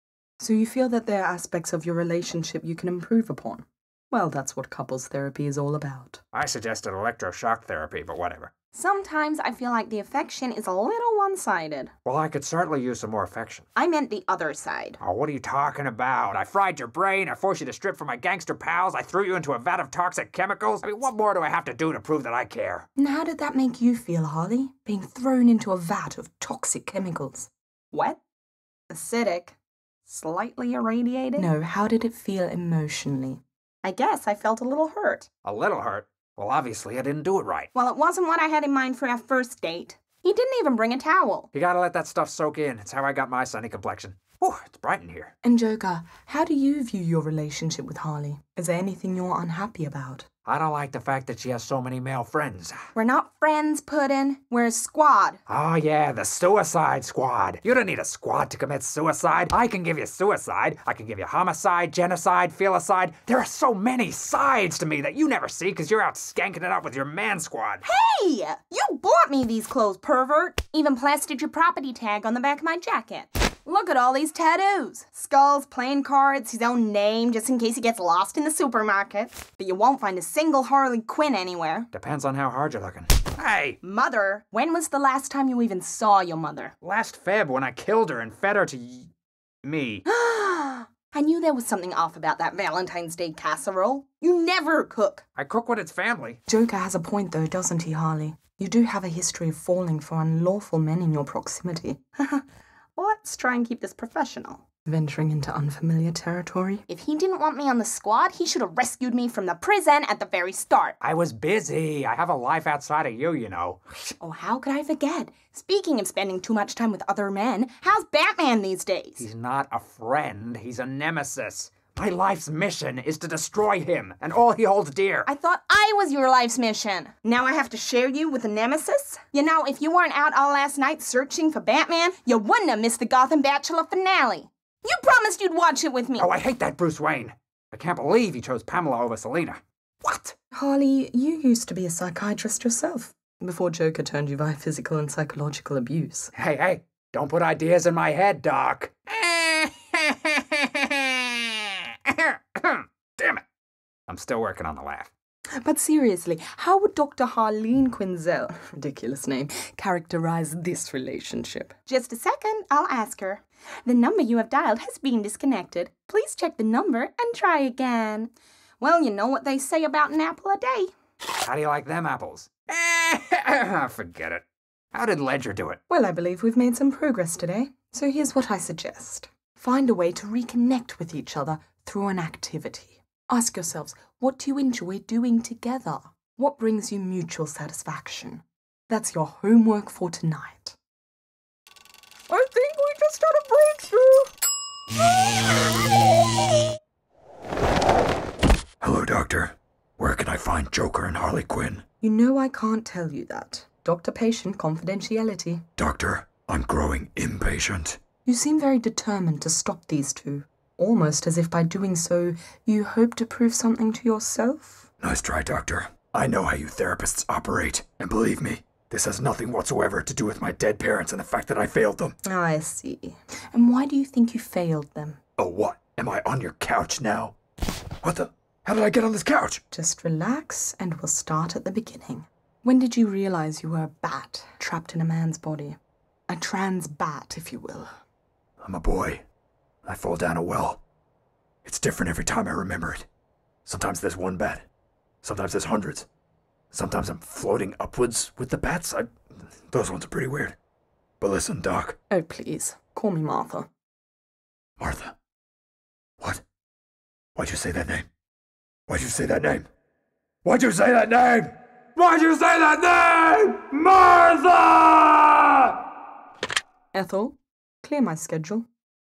Joker & Harley - Couples Therapy (Suicide Squad) - TOON SANDWICH
We send Joker and Harley Quinn to the best therapist in Gotham for some much-needed relationship counselling. What could possibly go wrong?